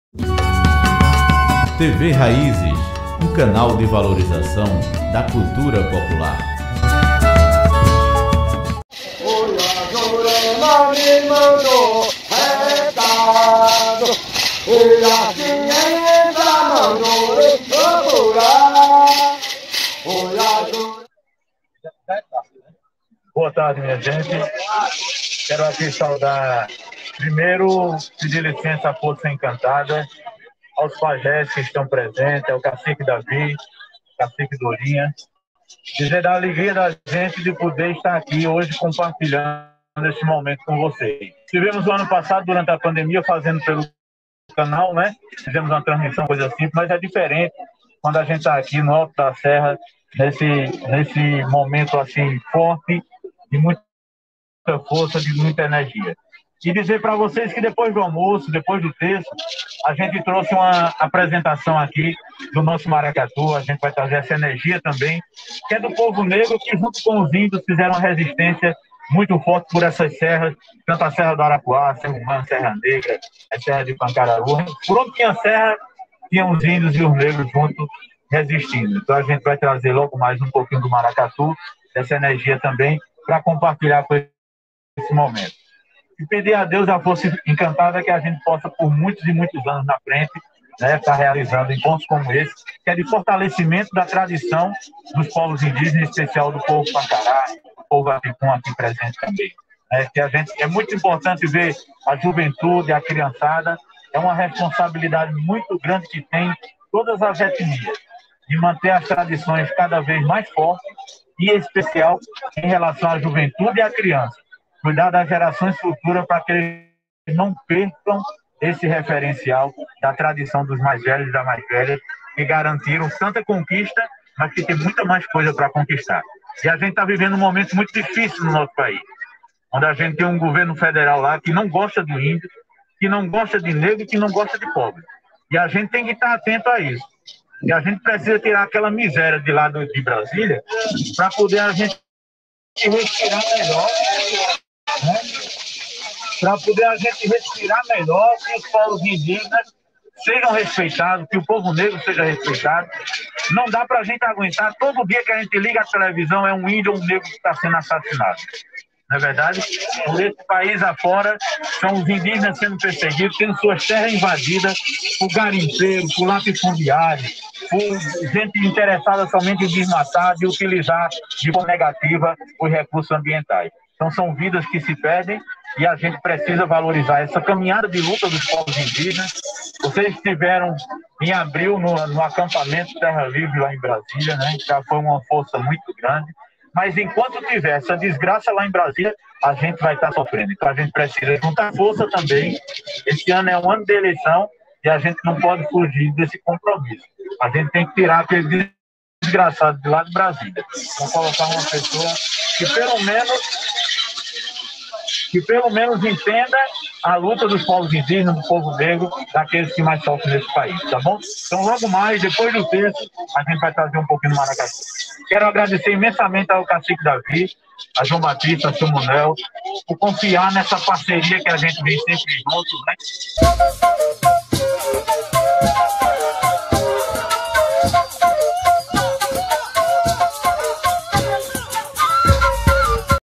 11º Encontro dos Caboclos de Aninha Gameleira
11º Encontro dos Caboclos de Aninha Gameleira, realizado no dia 26 de junho de 2022, na Aldeia Gameleira, na Serra do Arapuá, Território Indígena do Povo Pankará, município de Carnaubeira da Penha/PE.

Destaque para a fala de Libânio Francisco, Presidente do Instituto Cultural Raízes e Diretor da TV Raízes.